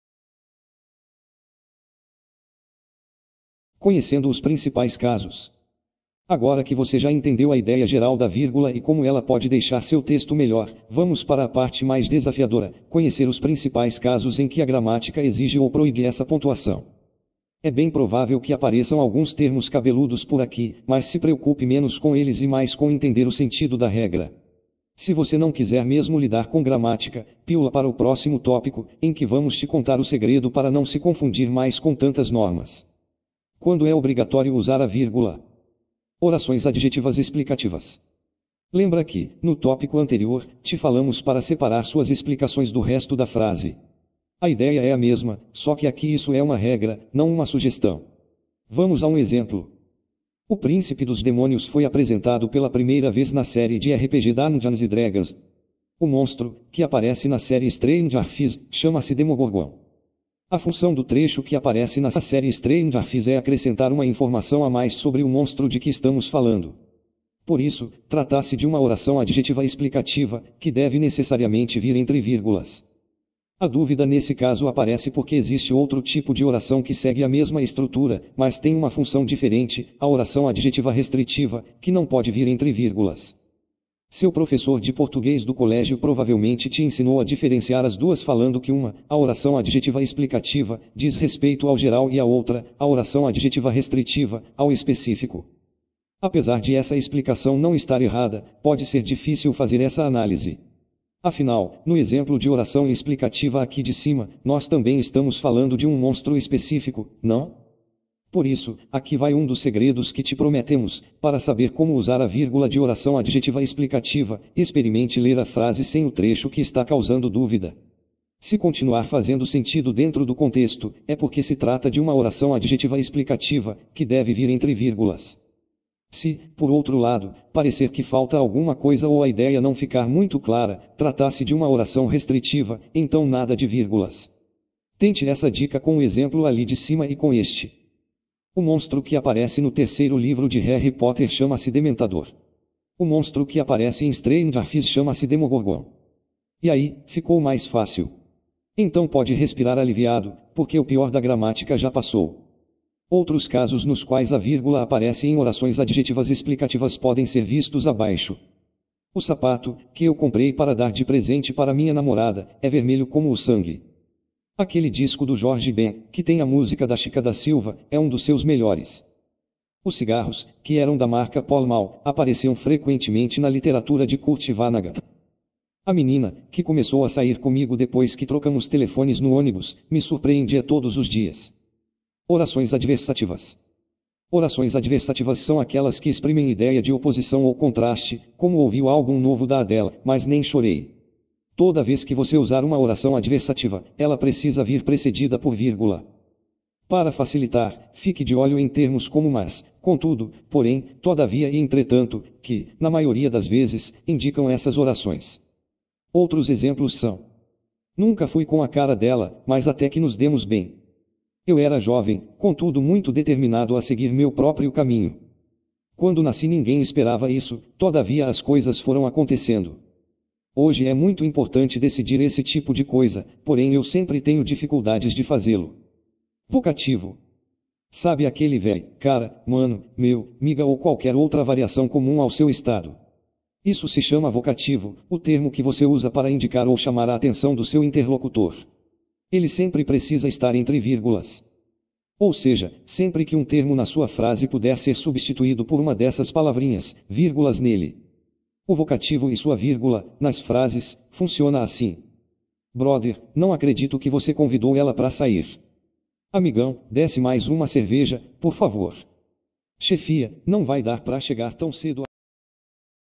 Guia completo de como usar a vírgula (PARTE 2) ''Conhecendo os principais casos''.
Neste vídeo, serão abordados exemplos, e também quando é obrigatório o uso da vírgula.